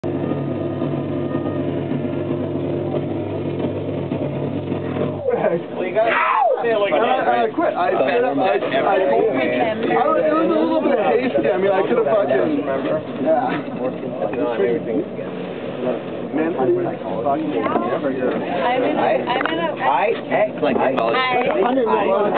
Coastal 16-02-08 Part 1
Live Coastal warm-up and volume test. The jacket comes off, courtesy of the Orkin Project, held February 16-08 in Victoria BC. Wanna see the set proper start with part 2!